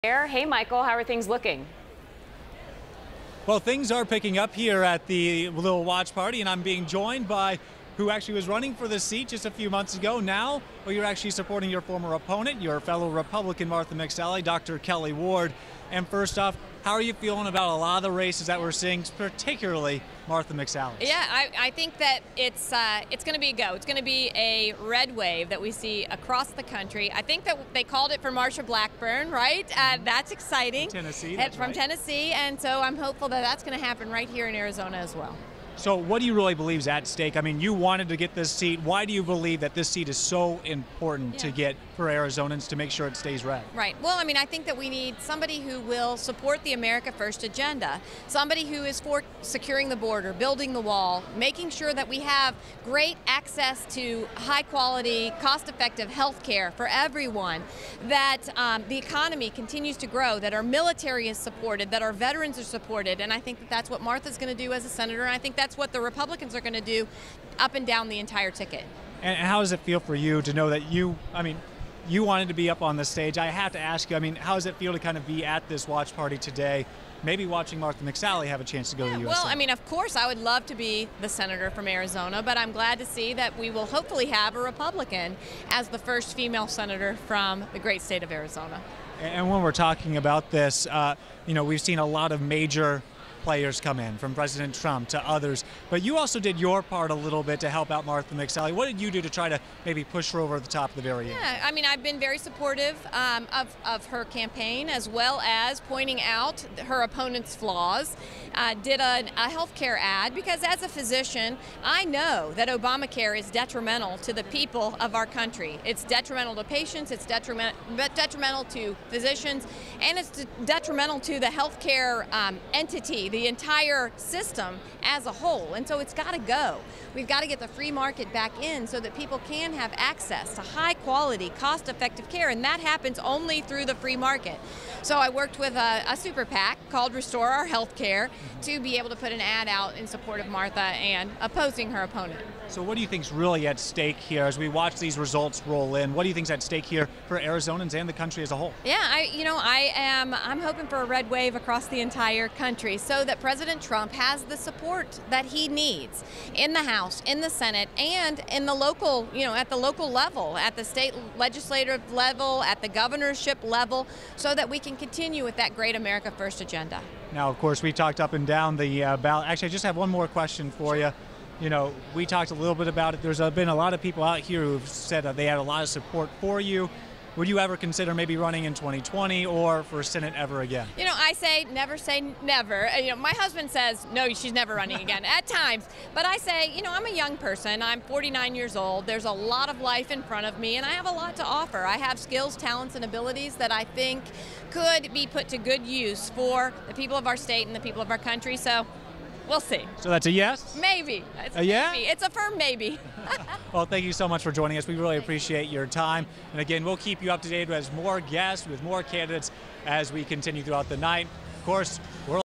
Dr. Kelly Ward supports Martha McSally at GOP watch party in Scottsdale
Dr. Kelly Ward was Martha McSally's challenger in the primary for the open U.S. Senate seat. Tonight, she is at the GOP watch party to put her support behind her former opponent.